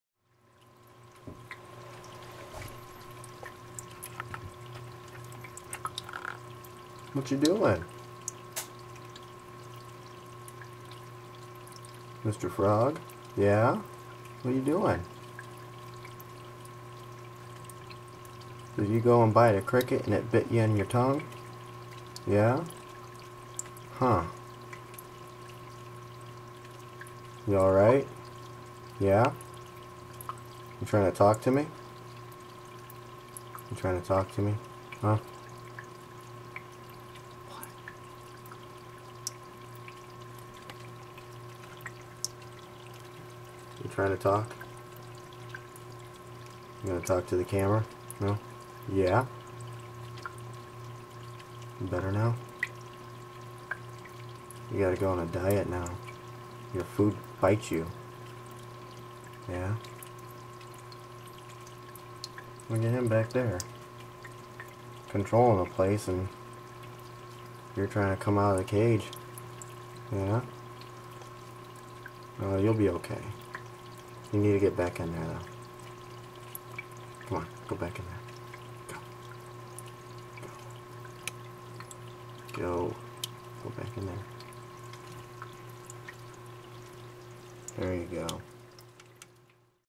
Mr. Red Eye Talks to the Camera
My Red Eyed Tree Frog gets up close and personal when a cricket he was going to eat bit him or something and he wigged out and spit him out... he wanted to let me know what happened I guess lol.